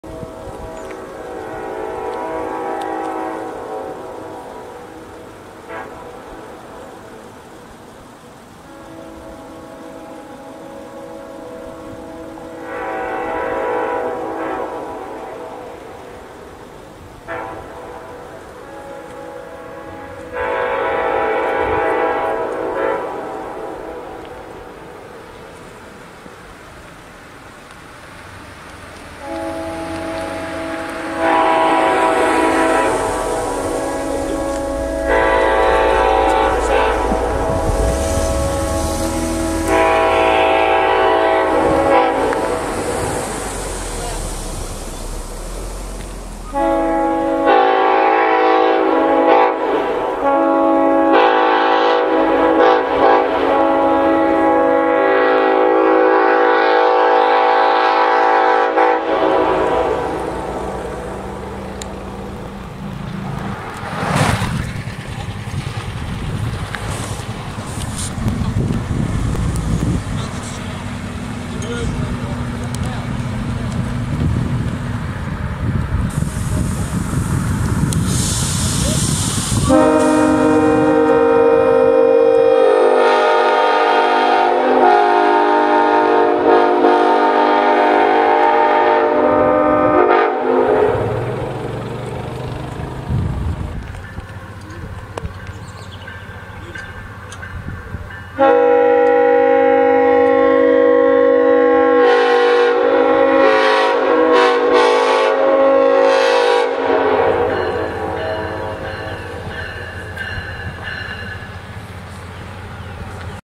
Nathan K5HLA
Jordan's K5HLA he recently acquired (again) out for a run at the Horshoe Curve in Altoona